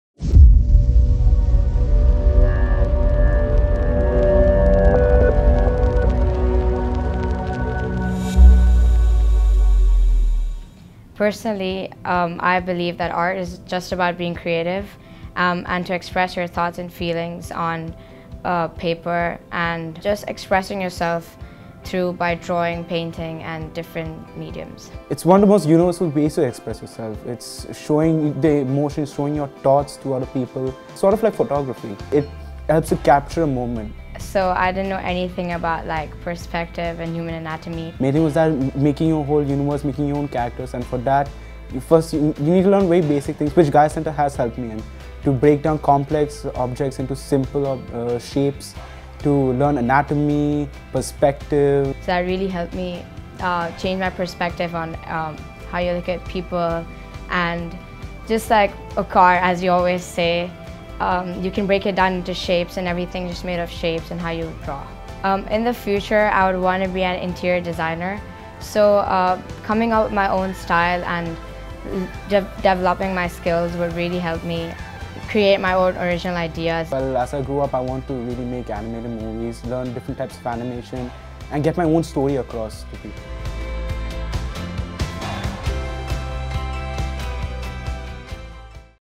Art @ The Gaia Centre
Our art students have a lot to say about their experiences and what they have learnt at The Gaia Centre. They discuss perspective, developing their concepts and ideas, and improving their skills in various aspects. Discover more.